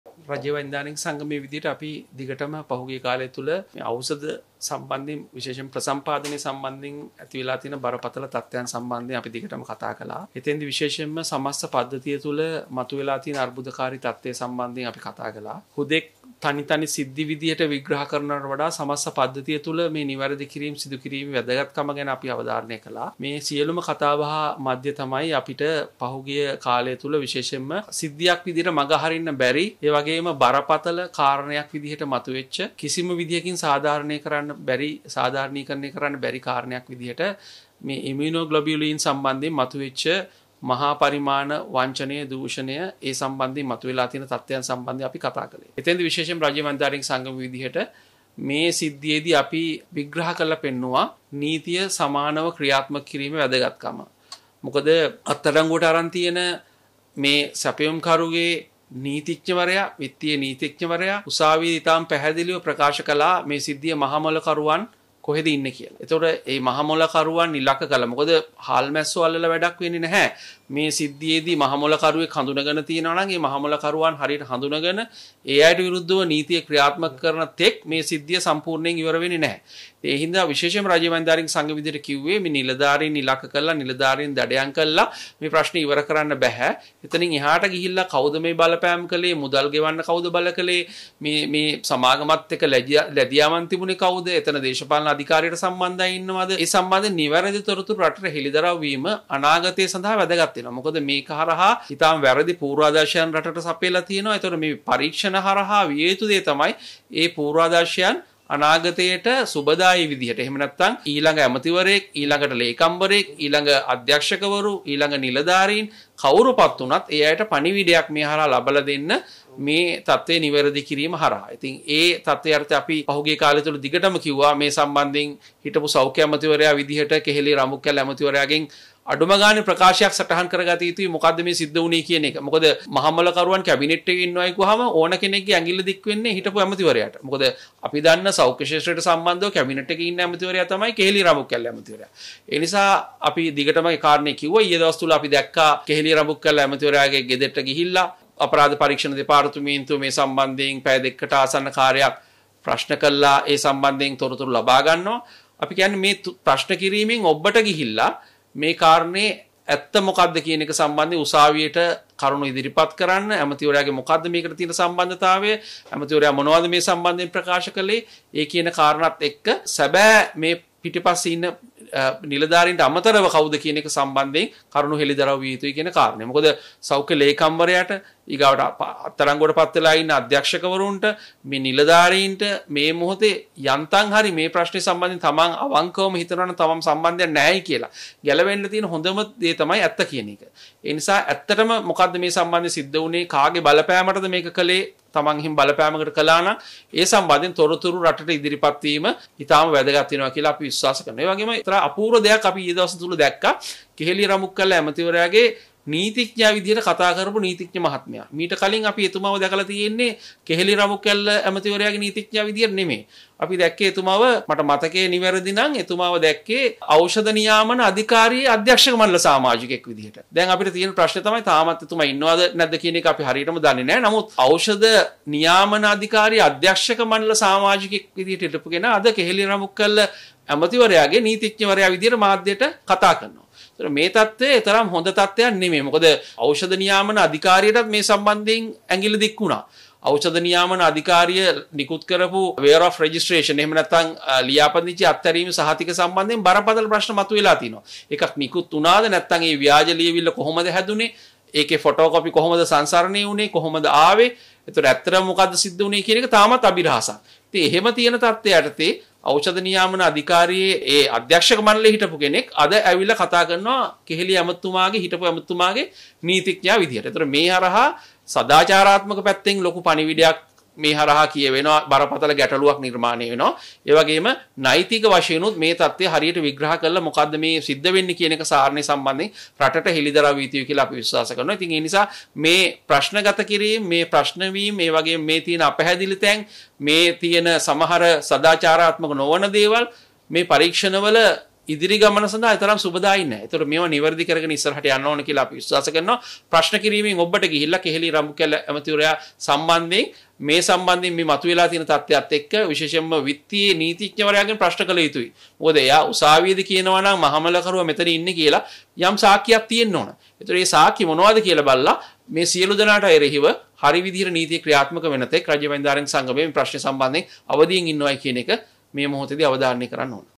ඖෂධ වංචාවේ මහමොළකරුවන් කවදා වෙනතුරු සැඟවෙයිද.?
Dr. Haritha Aluthge | Secretary GMOA | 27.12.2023